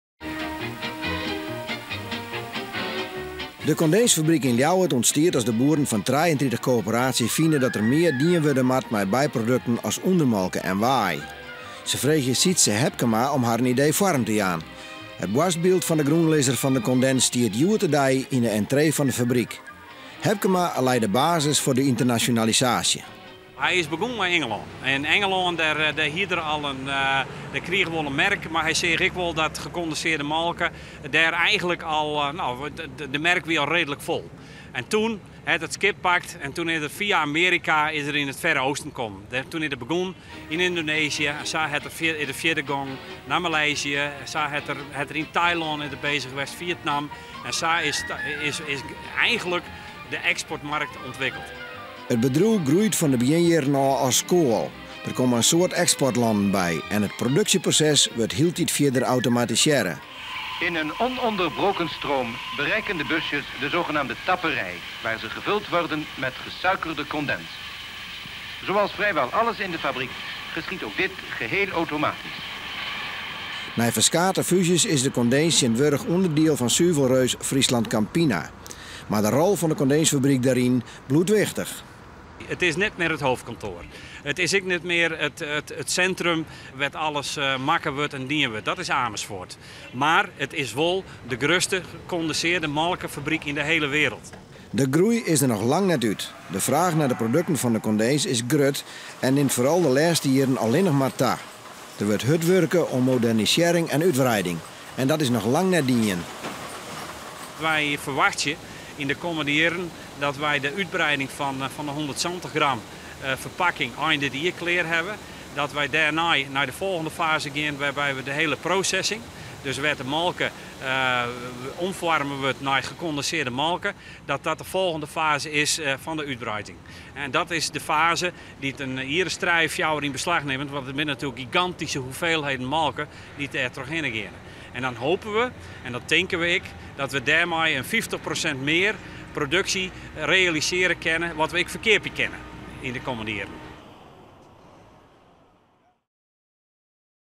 Molkfabryk klear foar takomst
It kondinsfabryk yn Ljouwert is klear foar nochris 100 jier. Dat seit de lokaasjedirekteur Langhout mei it 100-jierrich jubileum. Der wurdt 1 miljard liter molke deis ferwurke en dat is 9 % fan it totaal fan alle molke dy't binnenkomt by FrieslandCampina. Dêrmei is de rol fan it eardere Friesche Vlag noch altyd grut. De kommende tiid sil der foar miljoenen ynvesteard wurde sadat de kapasiteit nei 1,4 miljard liter deis giet.

Neffens de direkteur is der fanút de hiele wrâld grutte fraach nei goeie Nederlânske molke.